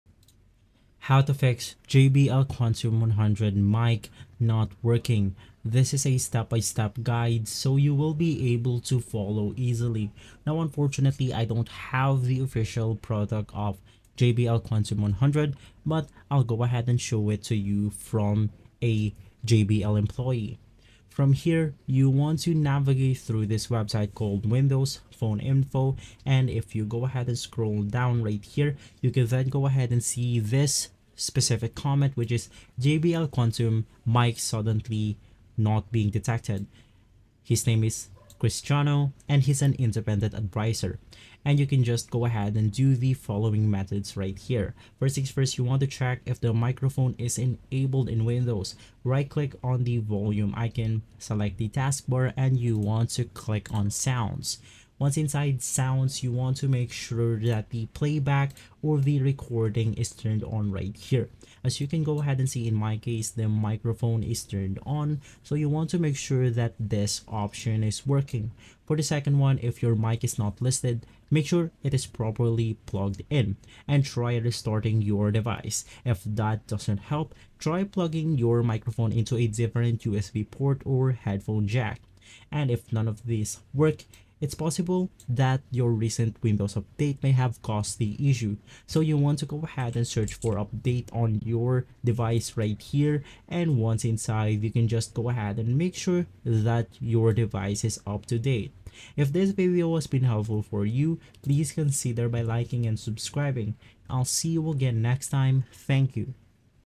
How To Fix JBL Quantum 100 Mic Not Working (Quick & Easy Tutorial)
Within this tutorial, you'll learn how to fix the JBL Quantum 100 mic not working with a quick and easy guide for 2024.

We’ll walk you through simple troubleshooting steps to resolve microphone issues, whether it's related to settings, connections, or hardware. This guide is perfect for anyone looking to get their JBL Quantum 100 mic working again without hassle.

Quick and easy steps to fix the JBL Quantum 100 mic not working
Learn troubleshooting methods to resolve common microphone issues in 2024
Ideal for gamers and users needing a fast solution for their headset mic

At the end of this tutorial, you’ll know how to fix your JBL Quantum 100 mic and get back to enjoying clear audio during your games or calls!